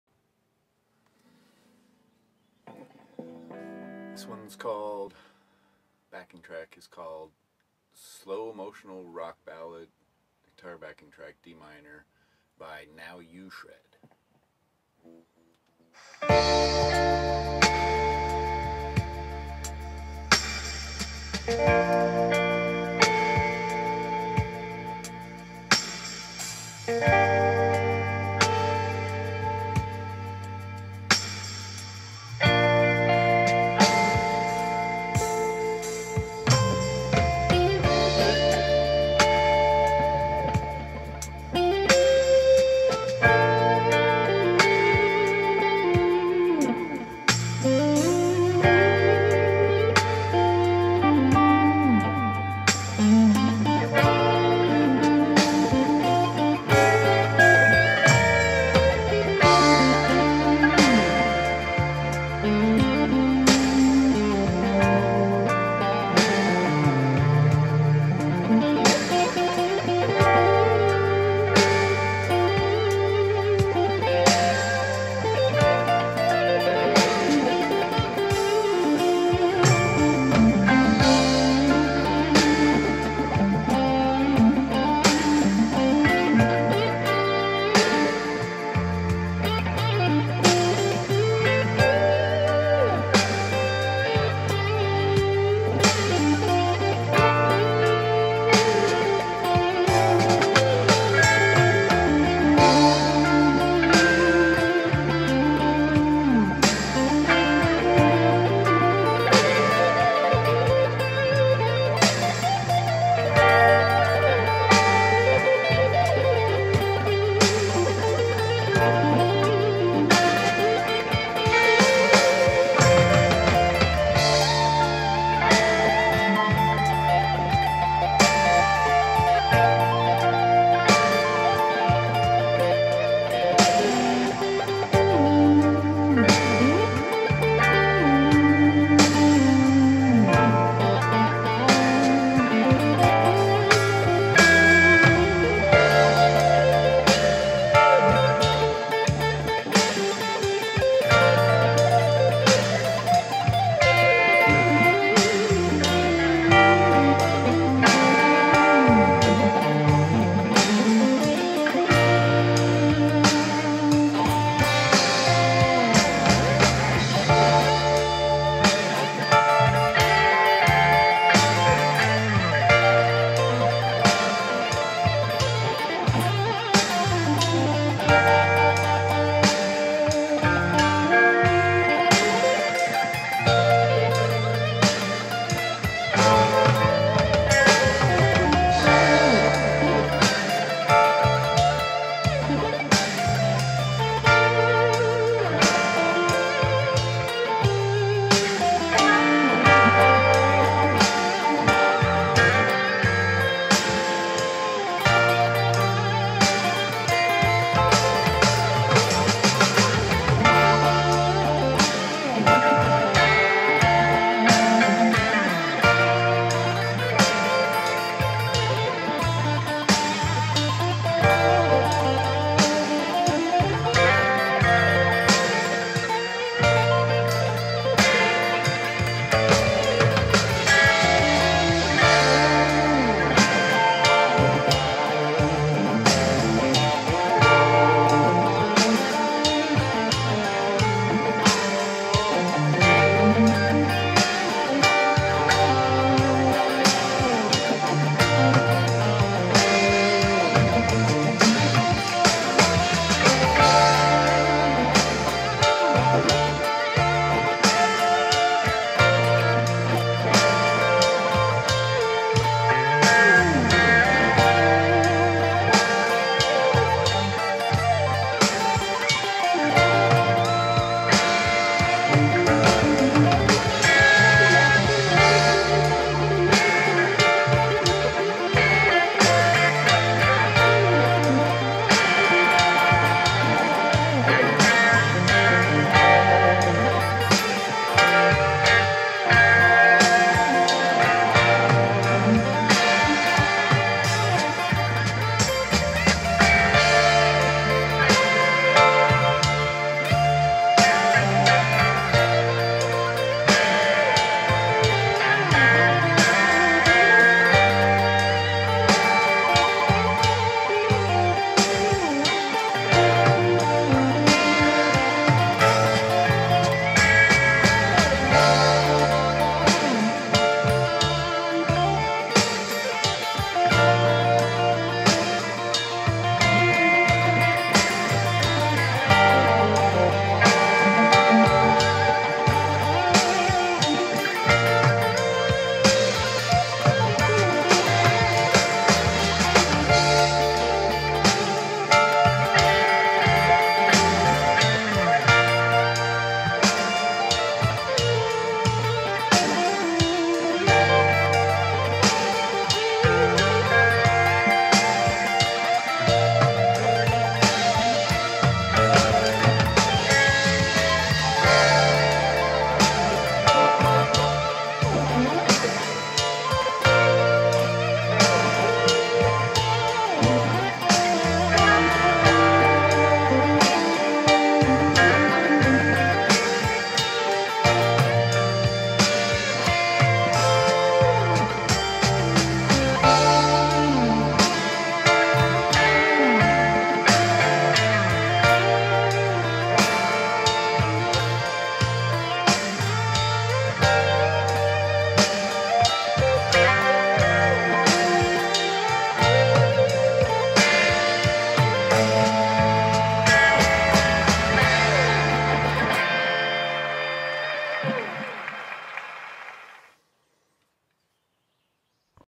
Eyes Closed Session 008
Here’s another in my practice sessions. This one was first thing in the morning, I can really hear the difference between this and the later in the day sessions when I’m more awake and warmed up. Worth it still. Sometimes you’ve got to be ready to perform in the morning.